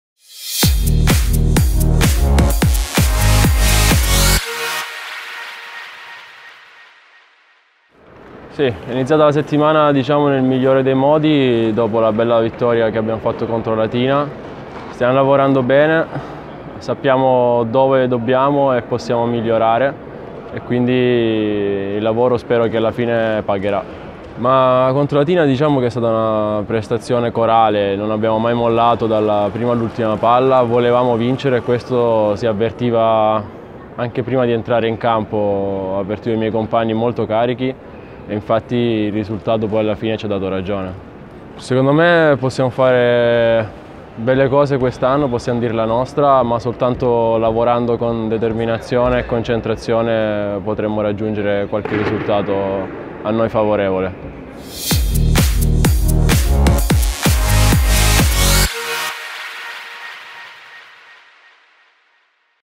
Marco Rizzo sulla settimana
Il libero del Gi Group Team Monza spiega la settimana di lavoro dei suoi in previsione della sfida in Emilia contro la Wixo Lpr Piacenza.